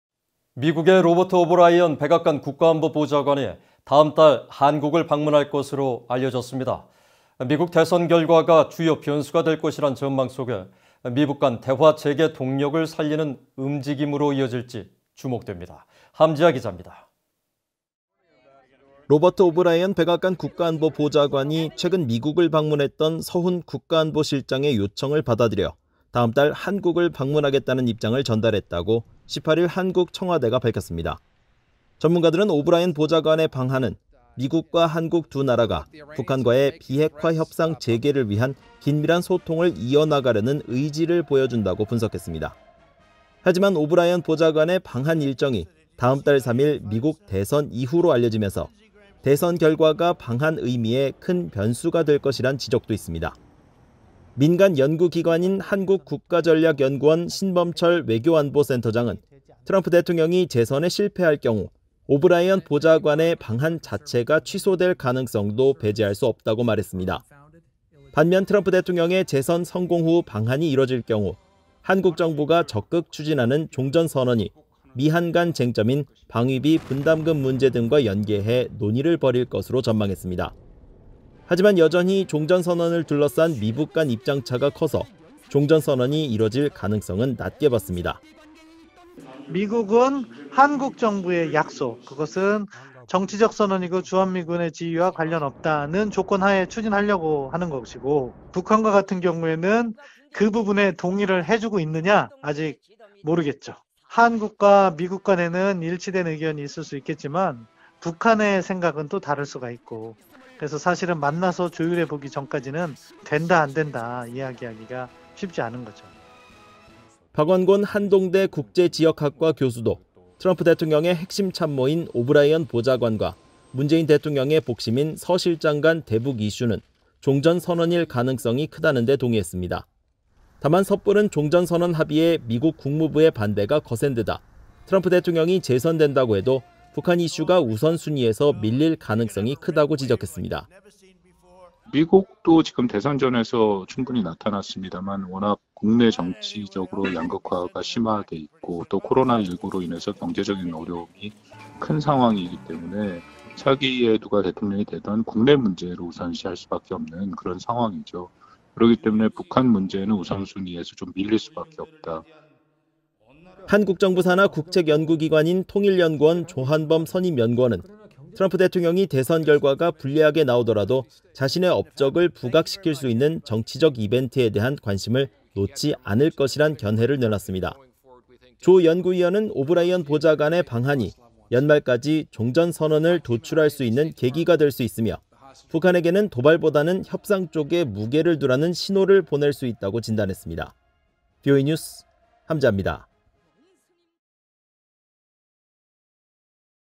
[VOA 뉴스] “오브라이언 ‘방한 예정’…미북 대화 ‘동력’ 변수”
미국의 로버트 오브라이언 백악관 국가안보보좌관이 다음 달 방한할 것으로 알려졌습니다. 미국 대선 결과가 주요 변수가 될 것이라는 전망 속에 도널드 트럼프 대통령의 임기 막바지 미북 간 대화 재개 동력을 살리는 움직임으로 이어질지 주목됩니다. 함지하 기자가 보도합니다. (영상편집: 강양우)